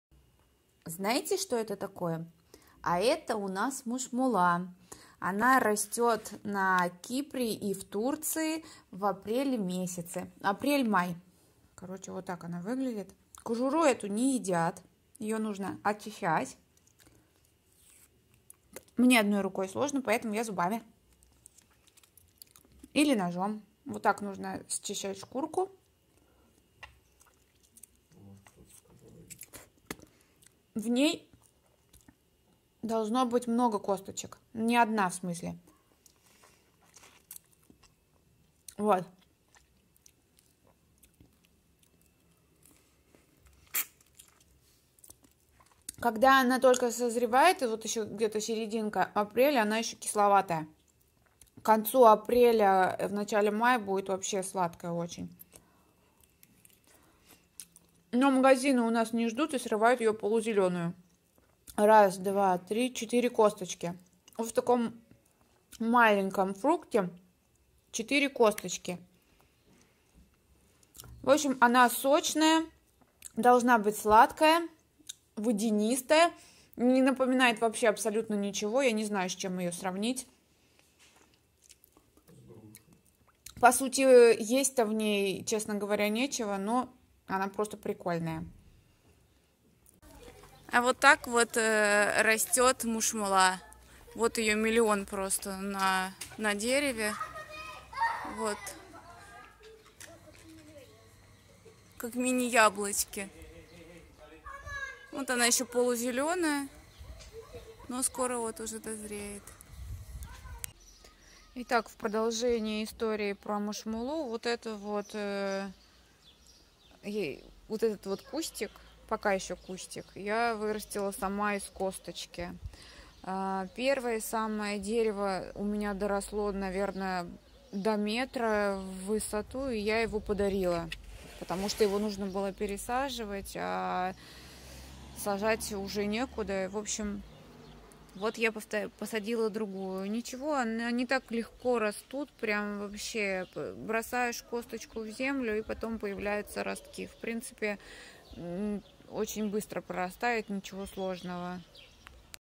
На Северном Кипре растет мушмула. Что это такое?
В апреле на рынках и в магазинах Северного Кипра начинает появляться удивительный фрукт. Мало кто про него знает из приезжих, и всегда снимают пробу, удивляясь, что же это все-таки такое? Желтый овальный фрукт, сочный, много косточек, непохожий ни на что!
А это мушмула (по тур. yeni dunya - новый мир). Влюбилась я в нее давно и с первой пробы. Даже сама не могу себе объяснить эту любовь - косточек много, да и есть особо нечего, но вот люблю и все!
Мушмула (по тур. yeni dunya) - колючий вечнозеленый субтропический кустик или дерево из семейства розоцветных. Кора ствола и ветви темно-серого цвета. Листья крупные, ланцетные, цельнокрайние, кожистые, глянцевитые, с нижней стороны войлочные. Цветки белые или светло-желтые, одиночные, с сильным ароматом.
Мушмулу едят в свежем виде, в районах произрастания из нее делают варенье, джемы, пастилу, мармелад, повидло, компоты, сок, а из семян - суррогат кофе.

✅ Оставляйте ваши комментарии с вопросами под видео.
✅ Предлагайте новые темы для видео, буду рада обратной связи.